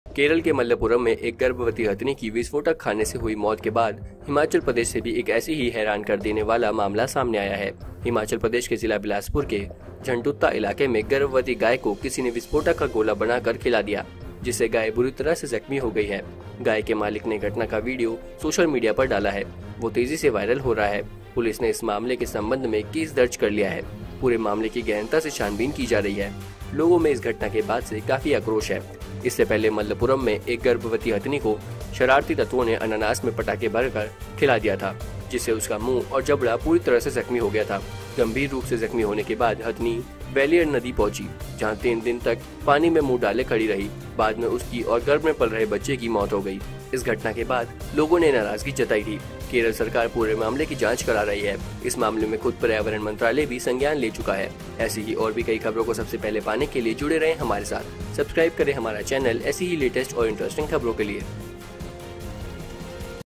केरल के बाद हिमाचल प्रदेश में हैवानियत, गर्भवती गाय को खिलाया विस्फोटक
India Trending News (ITN) : केरल के बाद हिमाचल प्रदेश में हैवानियत, गर्भवती गाय को खिलाया विस्फोटक

Edit By -